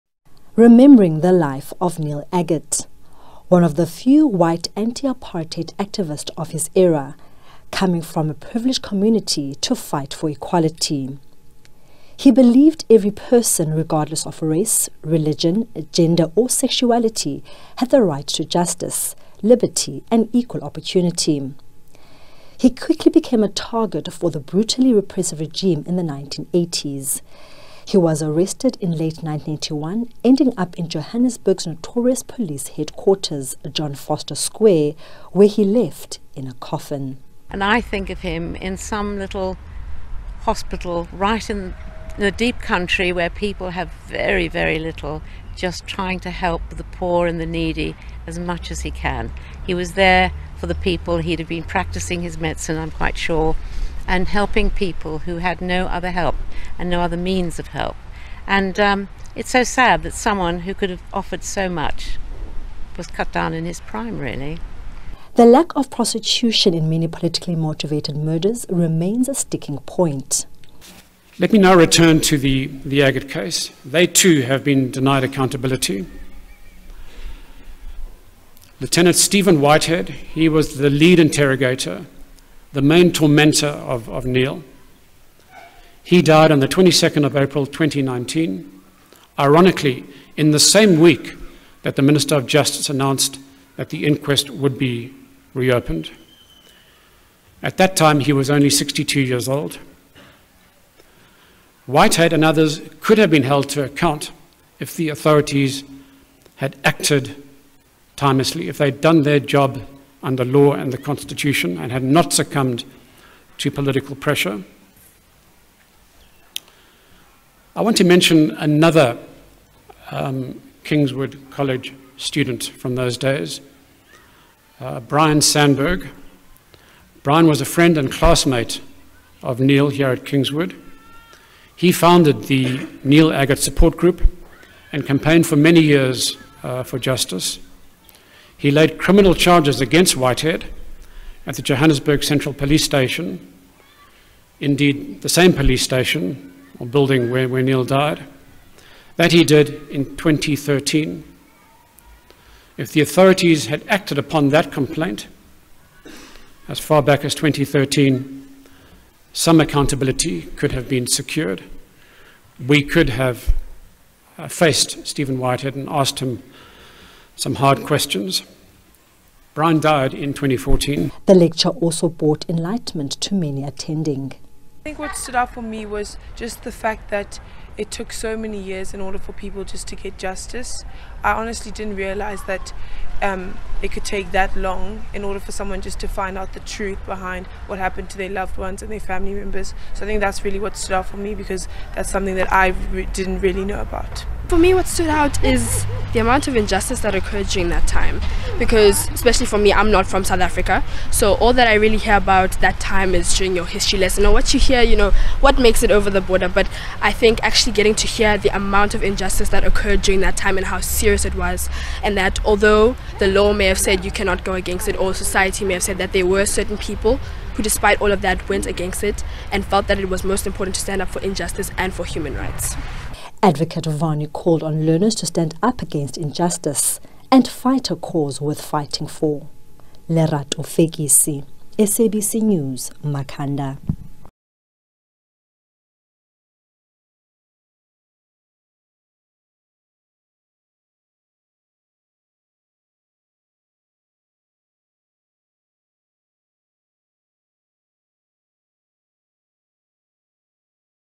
Annual Neil Aggett memorial lecture held in Kingswood College
The questions around interference in the prosecution of apartheid era political murderers has been raised again at the annual Neil Aggett memorial in Makhanda.

Advocate Howard Varney, who represented the Aggett family at the second inquest into his death, says political interference in these cases stonewalled the chance to get justice for leaders such as Aggett.

He delivered the annual Neil Aggett memorial lecture at Kingswood College.

Aggett died in police detention in 1982 after he was detained and tortured for days.